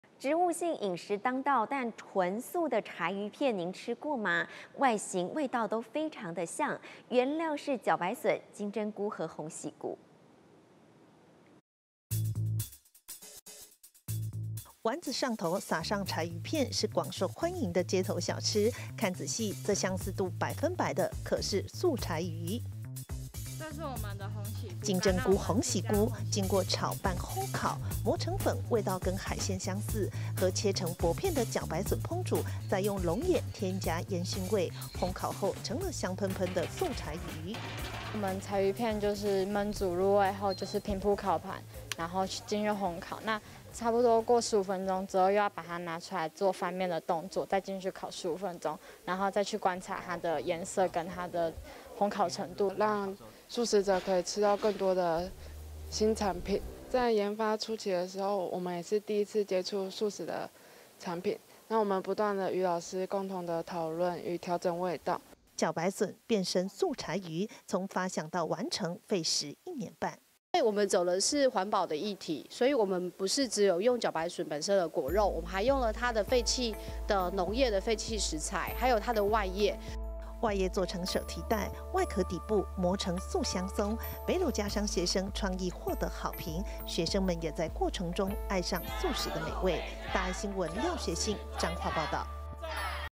筊白筍片創意料理 色香味不輸柴魚片｜大愛新聞
素食人口越來越多，提供素食者飲食新選擇，彰化北斗家商餐飲管理科學生，費時一年半，以筊白筍搭配金針菇粉、鴻禧菇粉，做成外觀、味道相似柴魚片的素食，創意獲得好評，在全國專題實作和創意競賽，拿下餐旅群創意組冠軍。